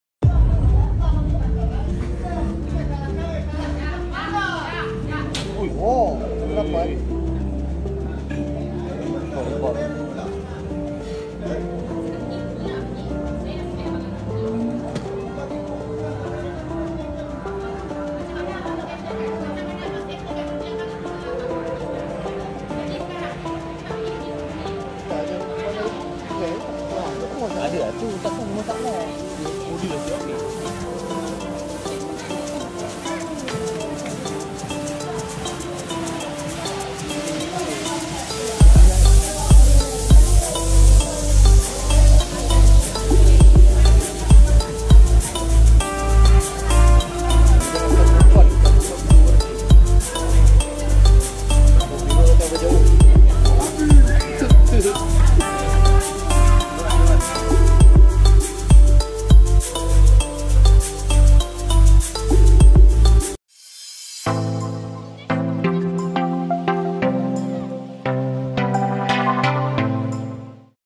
Polis Terima Laporan Kes 'Rider' Kena Pukul
Polis mengesahkan telah menerima laporan berkenaan kejadian seorang penghantar makanan yang dipukul di sebuah kafe semalam.